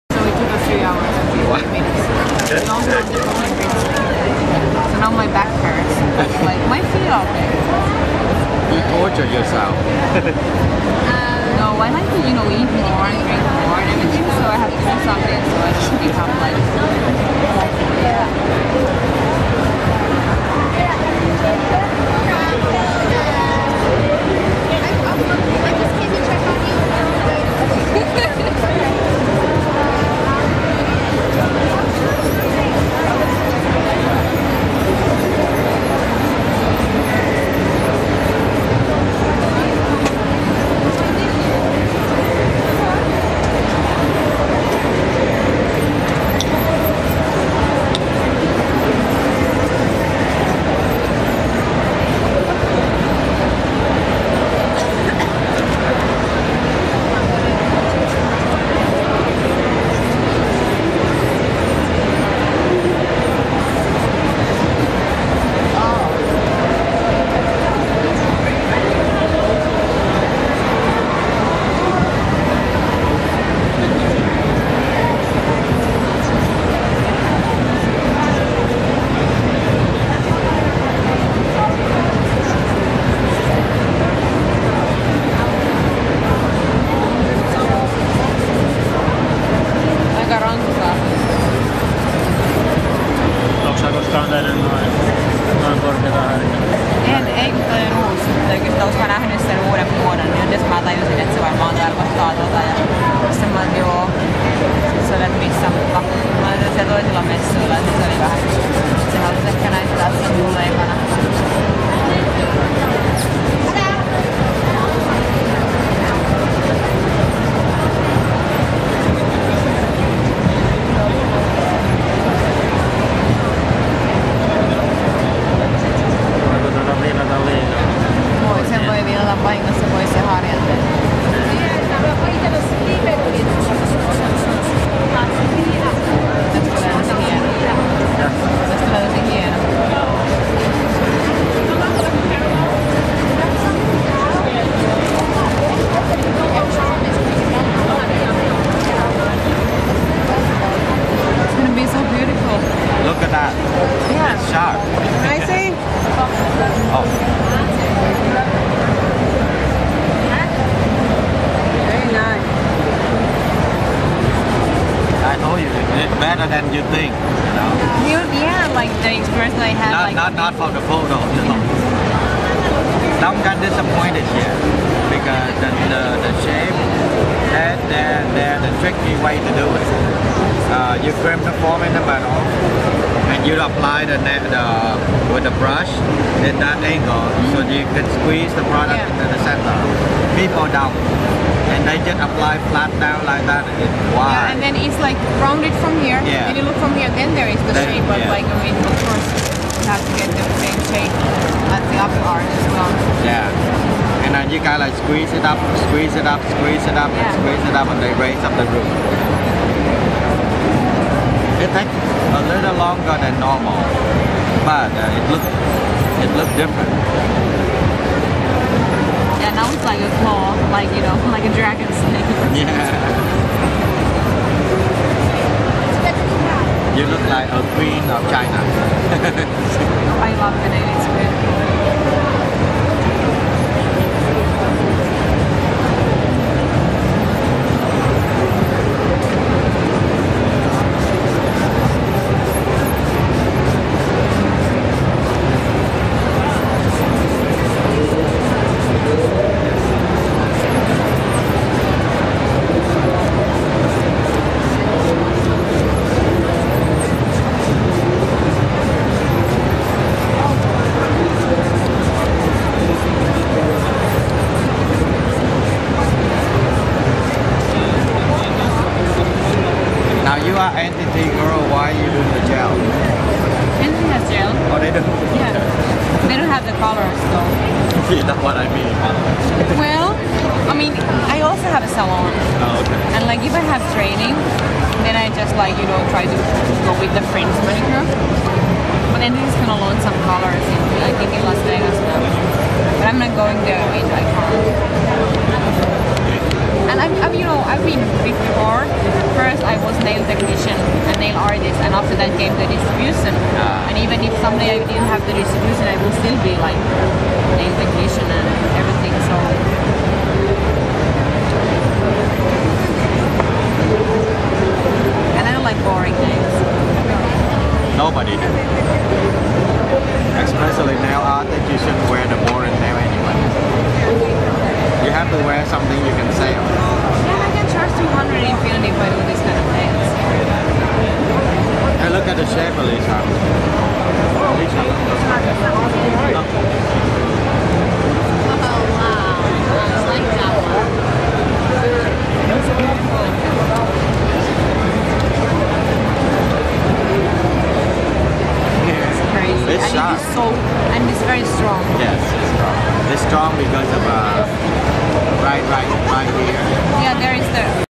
filing and buffing pyramid stiletto Edge nail part 2 by Trang Nguyen
IBS  trade show Jacob Javits NY 2009.Building of pyramid edge stiletto acrylic nail by  nail artist  Trang Nguyen.